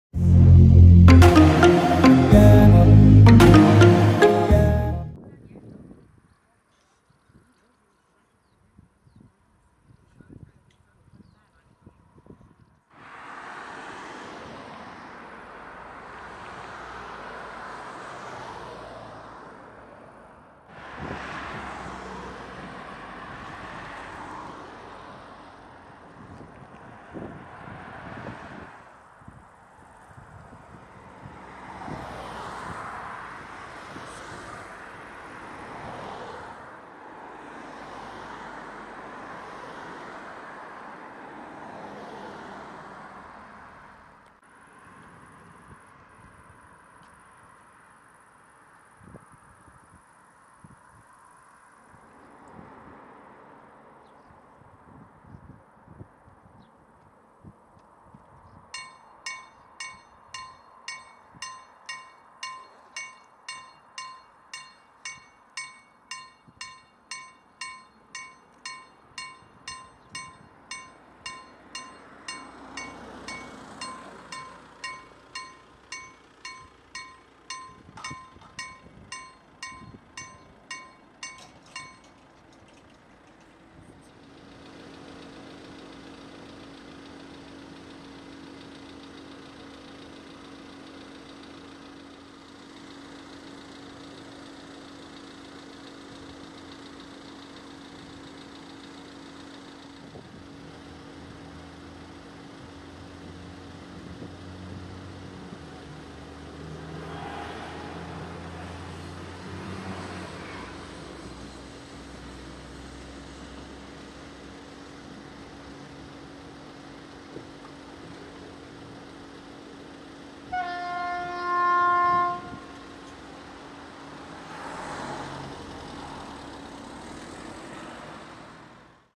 Przejazd kolejowy w Lesznie Grzybowie na ul. Osieckiej - SA132
Informacje o przejeździe:

Numer przejazdu: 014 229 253
Kategoria przejazdu: A
Typ automatyki: SPR-1
Sygnalizatory: 4x DS-T
Rogatki: 4x JEGD-50
Sygnał akustyczny: 1x KLD-4003
Momenty filmu:
0:00 Intro
0:05 Prezentacja przejazdu
0:51 SA132-003 Kolei Wielkopolskich relacji Ostrów Wielkopolski - Leszno odjeżdża ze stacji Leszno Grzybowo
Data nagrania filmu: 17 lipca 2022 r.